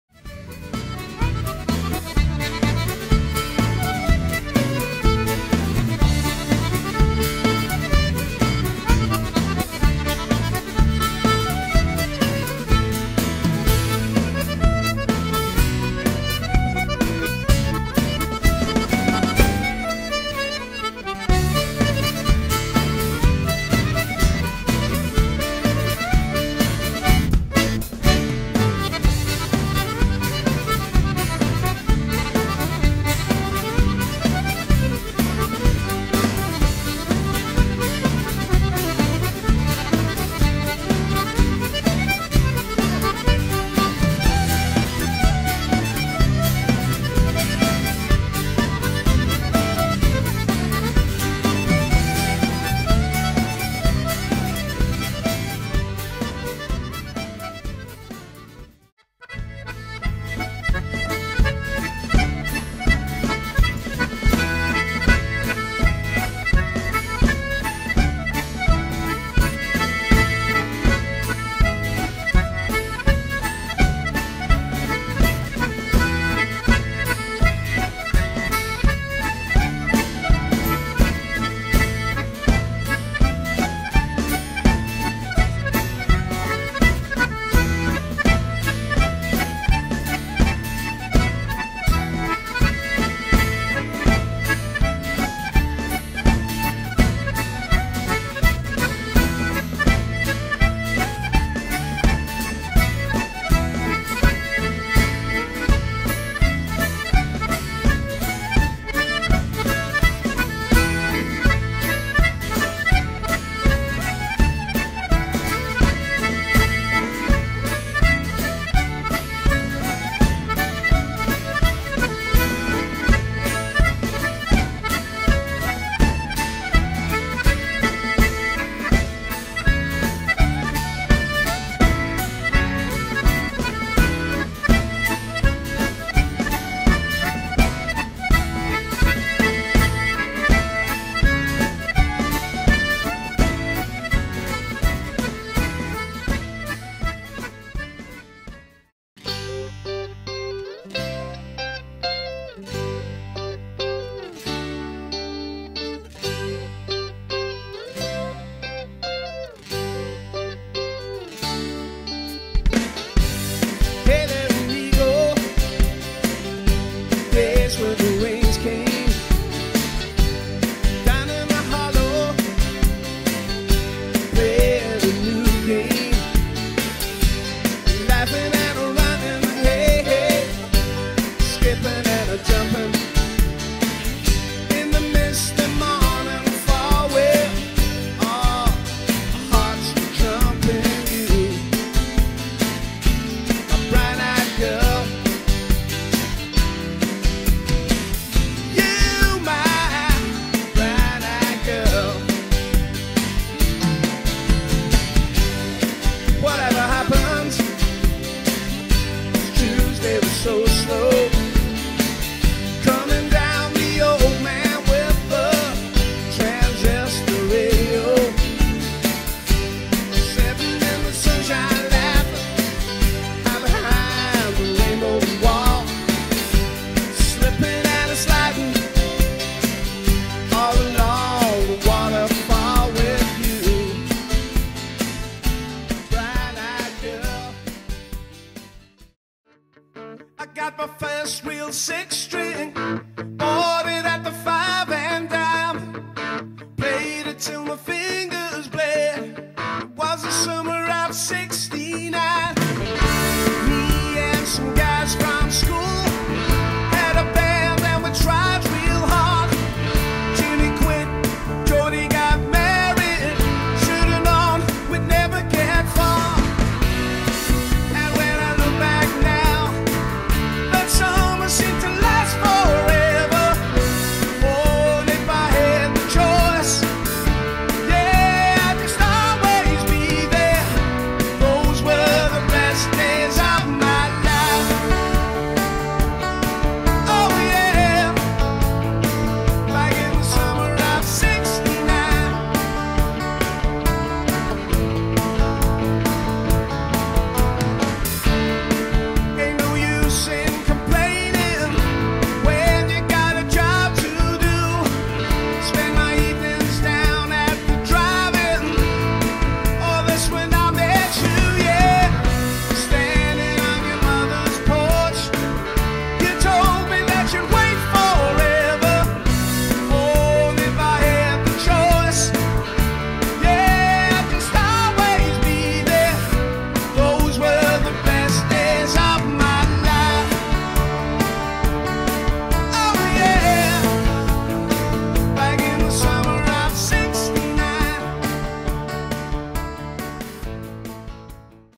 Scottish ceilidh band Ceilidhdonia
The thing that unites CeilidhDonia is their collective love of making music.  Formed in 2007, this exciting band of talented multi-instrumentalists, have travelled far and wide. As well as gigging all over the UK, they have performed in many countries including Libya, Mexico and Portugal, to name but a few.